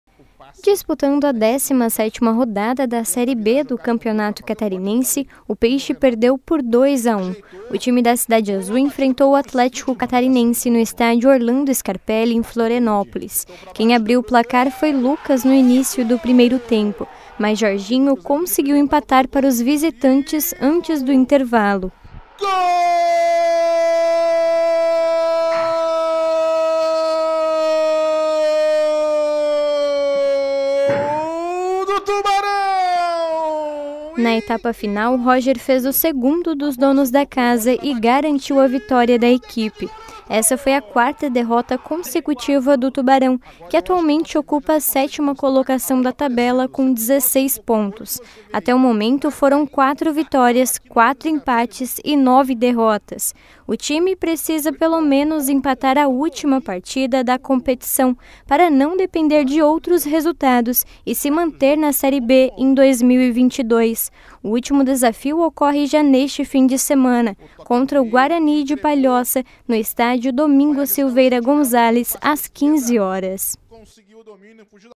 PEIXE PERDE FORA DE CASA EM MAIS PARTIDA DO CATARINENSE – 09/09/2021
O atlético Tubarão sofreu mais uma derrota na série B do campeonato catarinense. O jogo terminou em 2 a 1